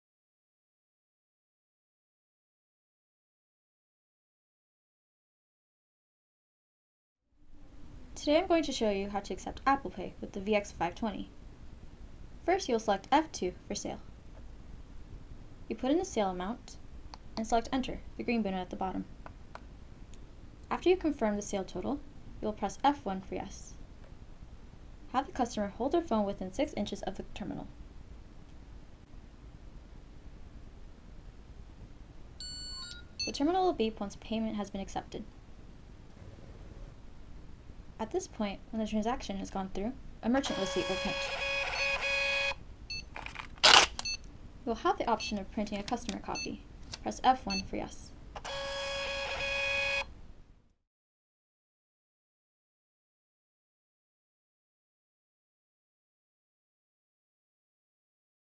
VX520- How to Process Apple Pay
This video shows you how to Process an Apple Pay Transaction on your VX520 Terminal.